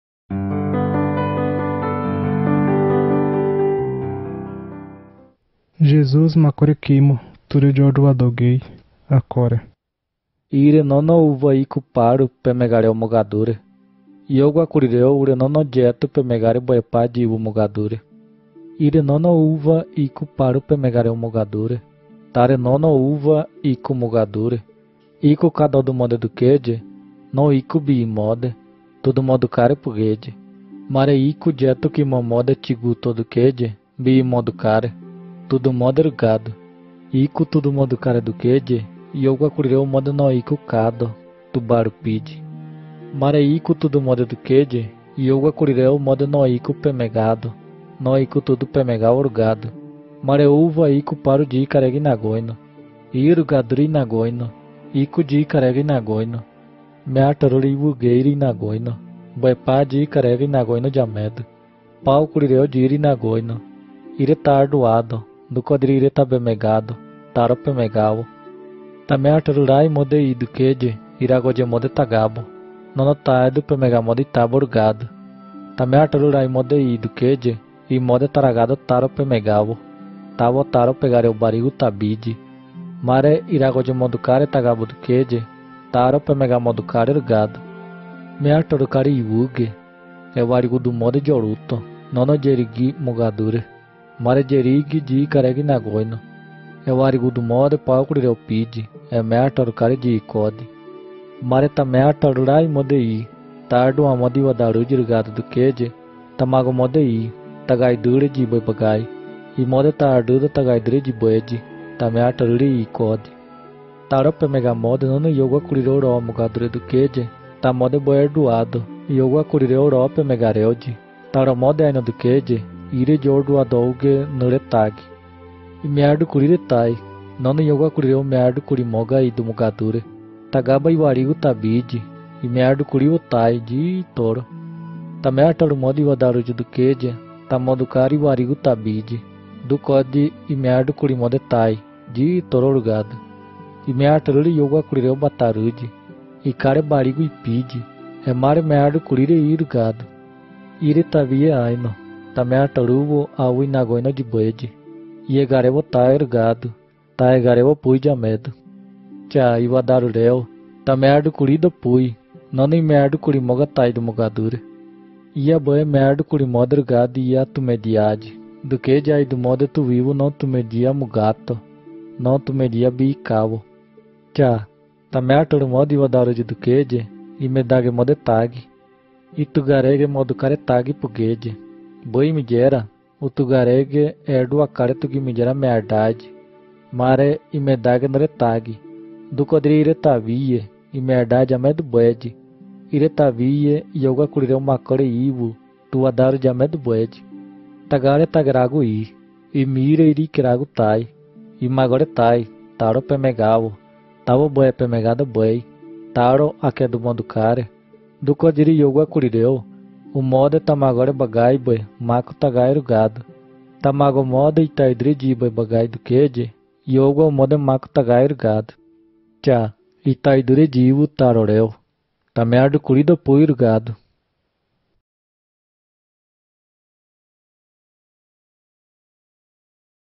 Borôro - John 15:1-17 - The Vine and the branches [bor]
Scripture Videos from the Gospel of John in Borôro of Brazil. [bor]